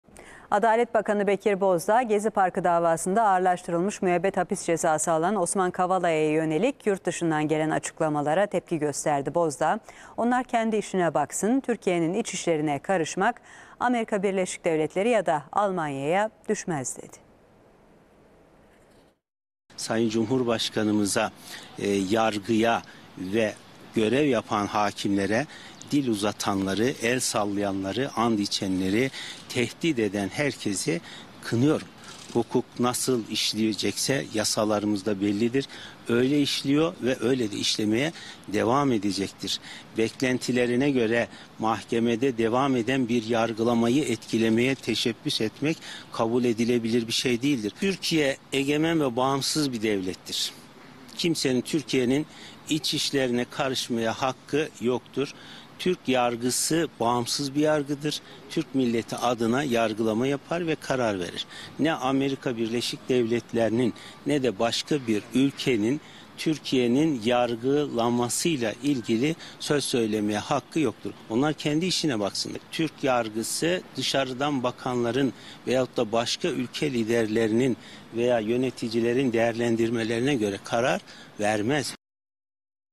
Adalet Bakanı Bekir Bozdağ'dan Gezi Davası kararı sonrası tehditler savuran muhalefete sert tepki
Adalet Bakanı Bekir Bozdağ Gezi Parkı davasında alınan kararlar hakkında açıklamalarda bulundu.

Son dakika haberi: Adalet Bakanı Bekir Bozdağ, Başkan Erdoğan'ı tehdit eden CHP Grup Başkanvekili Özgür Özel'e, "Kimsenin seçilmiş Cumhurbaşkanına parmak sallamaya hakkı da, haddi de yoktur." sözleri ile tepki gösterdi.

Bozdağ, hakimleri, 'Hesap verecekler' diyerek tehdit eden bir diğer CHP Milletvekili Sezgin Tanrıkulu'ya da sert çıktı, "Türkiye hukuk devletidir. Kimse yargının üstünde değildir. Cumhurbaşkanımıza ve hakimlerimize dil uzatanları kınıyorum." ifadelerini kullandı.